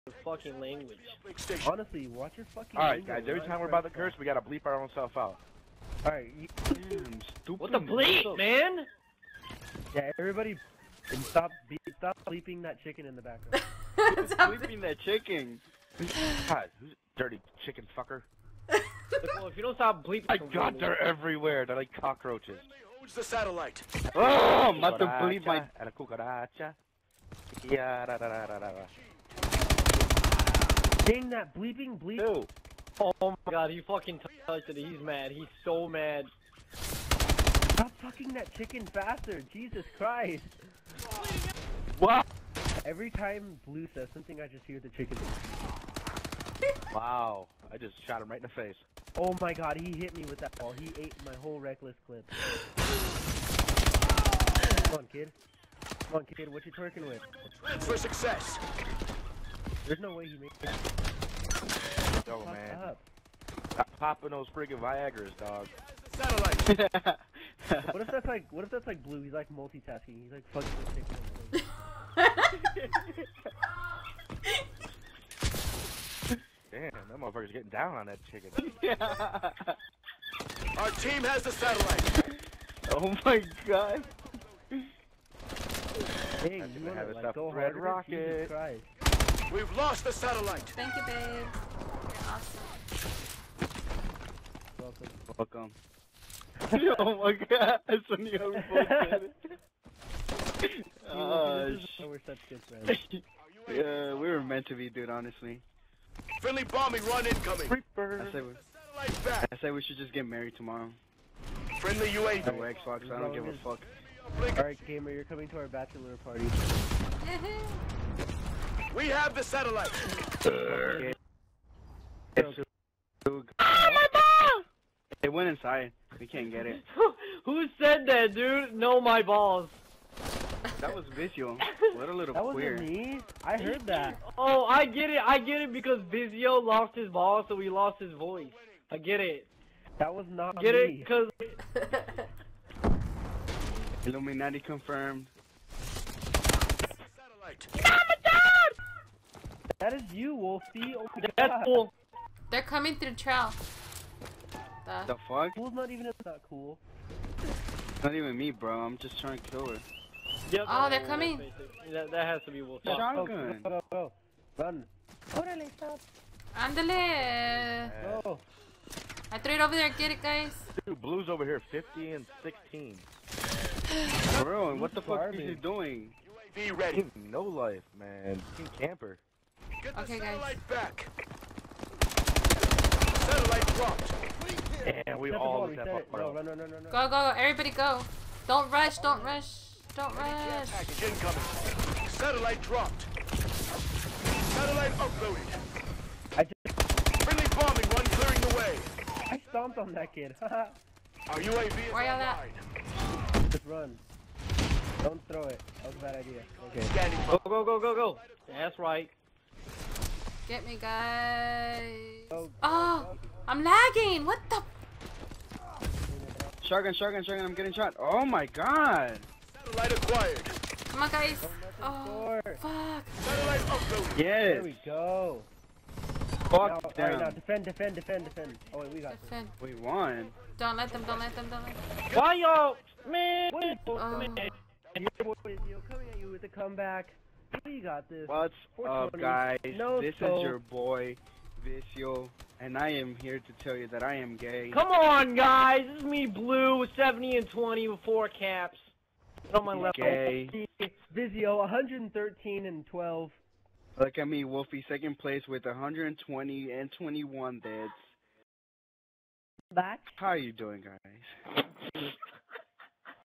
[GM3R] V'S [FaDe] with a come back (with a Chicken Fker)
Mexico City Clan wars was fun we laughed most of time. and also kick major butt